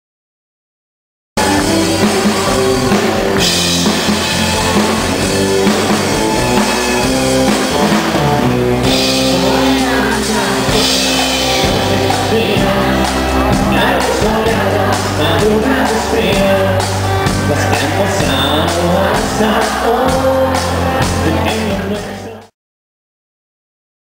www VaKeRos cOm Festival Country Chih 09
Presentacion de Ocho segundos y caballlo dorado en la presentaciuon de magno evento del festival country chih 2009